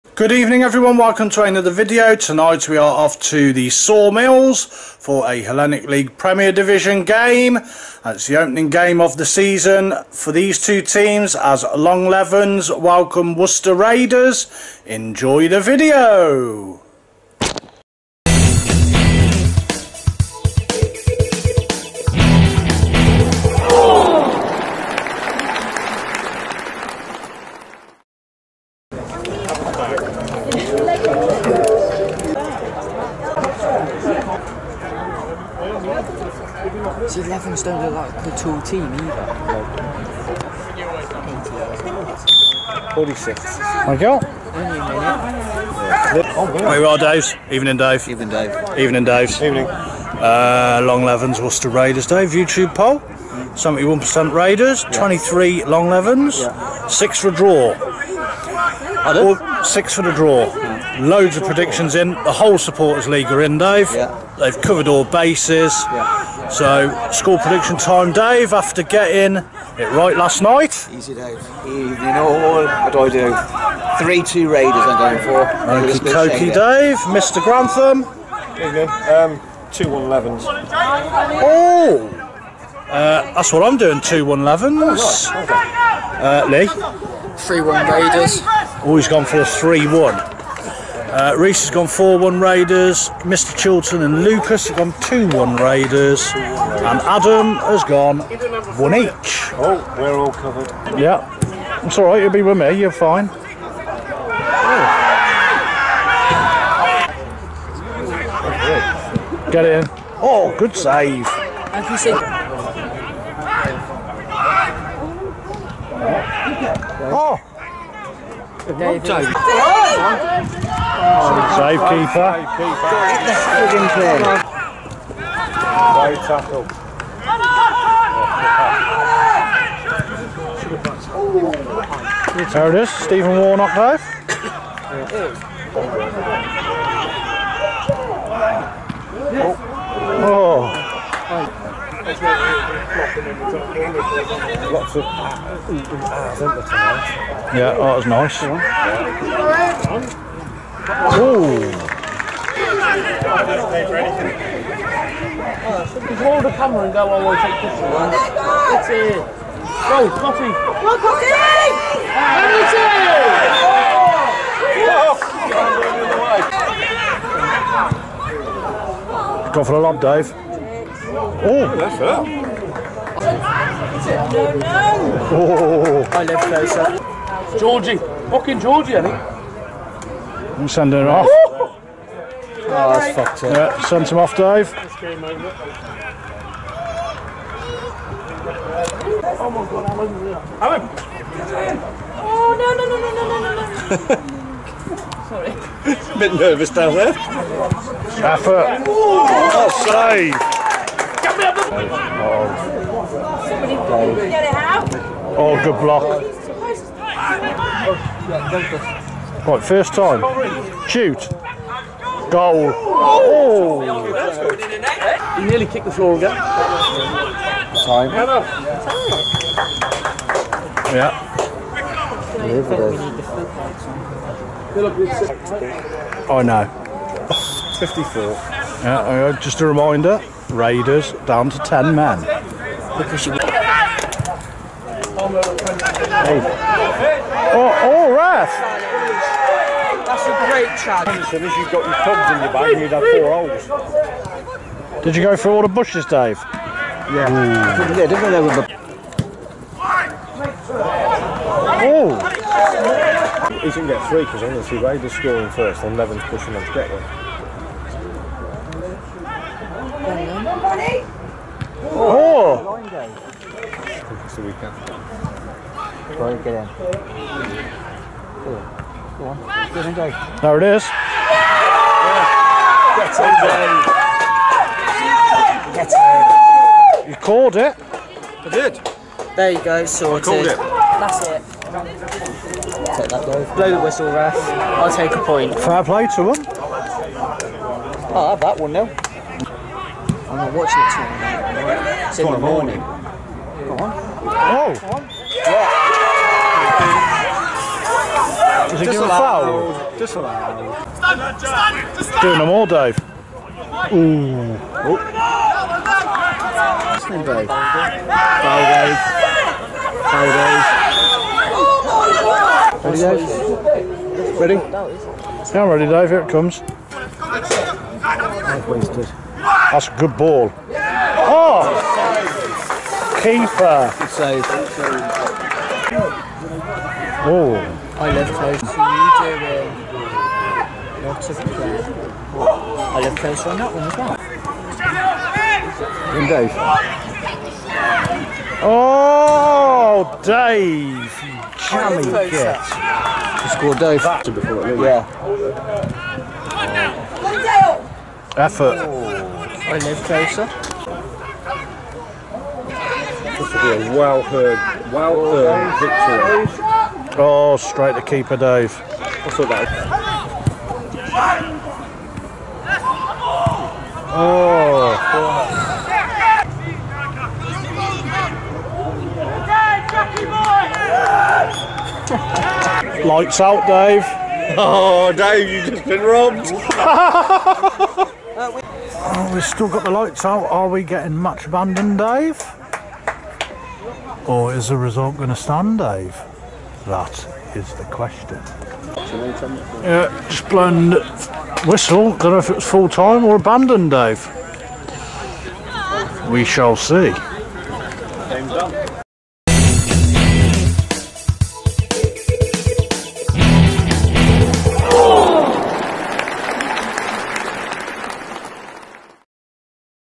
| Longlevens AFC v Worcester Raiders |
Wednesday 31st July.

The Saw Mills 🏟️ .

Hellenic League Premier Division.

Longlevens AFC v Worcester Raiders FC.